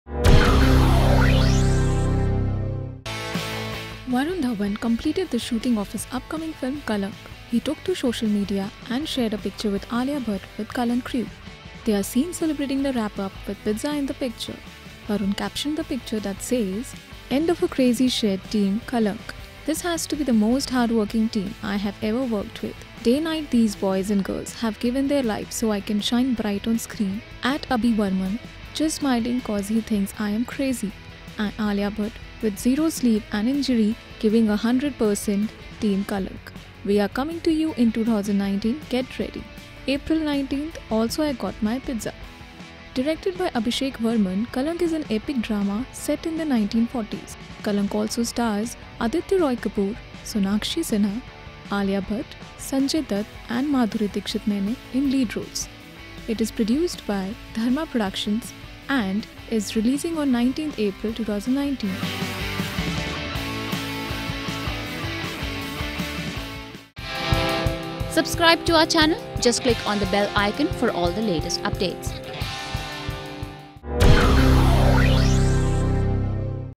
Varun wraps up 'Kalank' with Pizza party
Varun Dhawan completed the shooting of his upcoming film Kalank' .He took to social media and shared a picture with Alia Bhatt with Kalank crew. They are seen celebrating the wrap up with pizza in the picture.